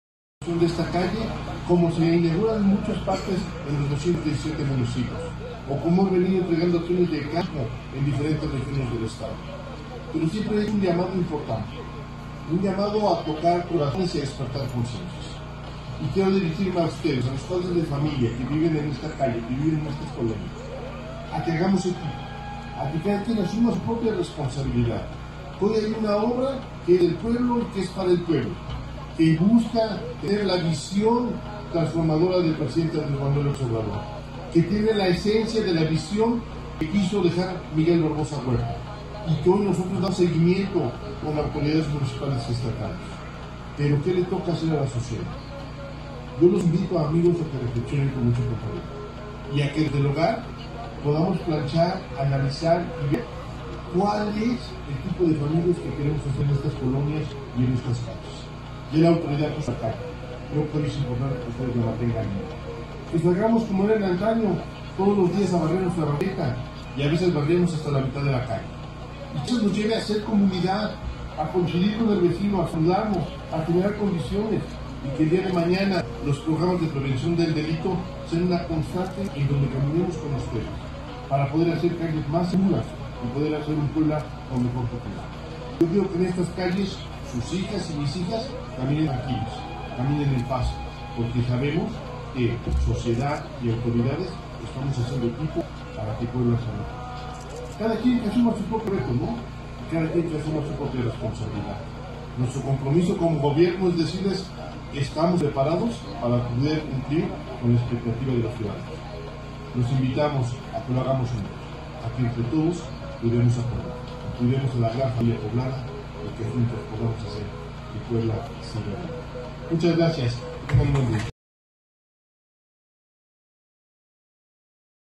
Sergio Salomón reiteró que seguirá la idea de Miguel Barbosa para mantener la autonomía de la BUAP
Frente a la rectora Lilia Cedillo, el gobernador de Puebla, Sergio Salomón reiteró que dará seguimiento puntual al legado que dejó el exmandatario, Miguel Barbosa para mantener la autonomía de la BUAP.